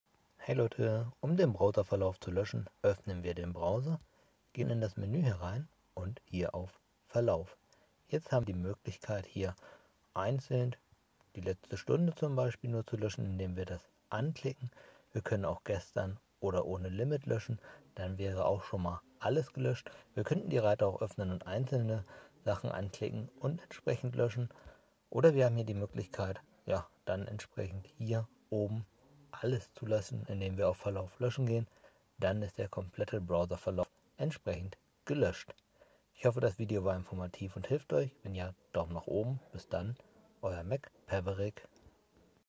Samsung Handy Browserverlauf löschen - Samsung Internet Browser Verlaufleeren - Anleitung ✅
Ich zeige euch, wie Ihr den Verlauf vom Browser beim Samsung Smartphone
löschen und leeren könnt.